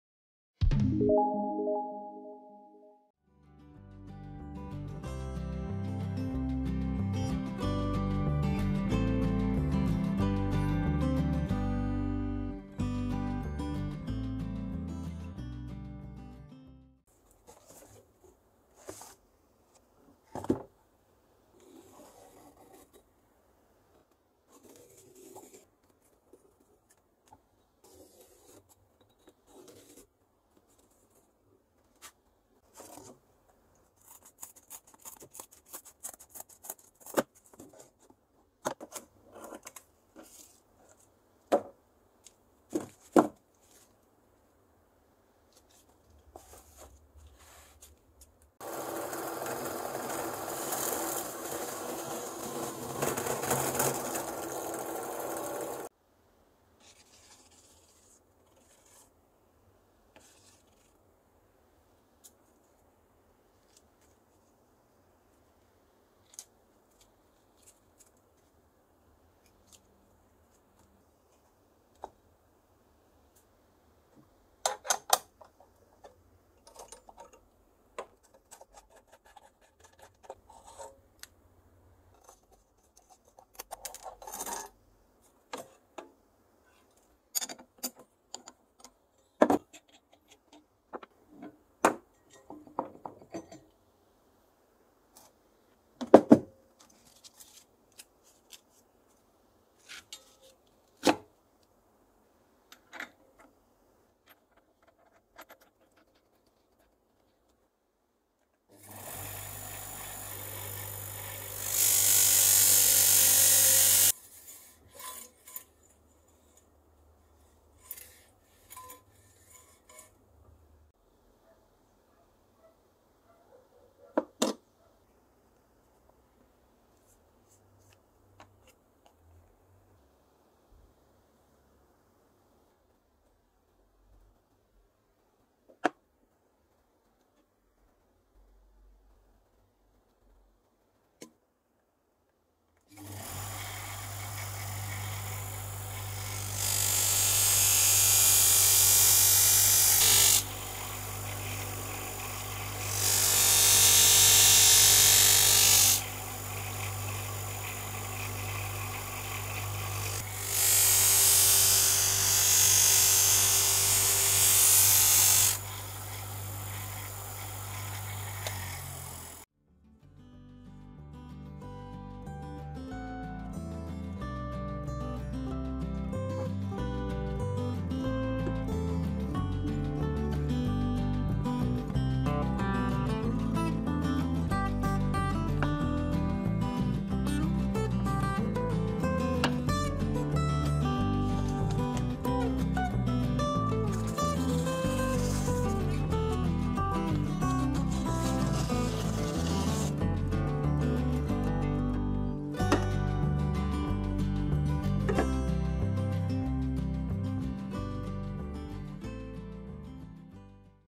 Only a Few Believe me !! Sharpen Dull Knives with Cardboard !!
This is an amazing method to sharpen a Knife to razor sharp with no special tools. Razor-Sharp knife sharpening can be easy with the correct method !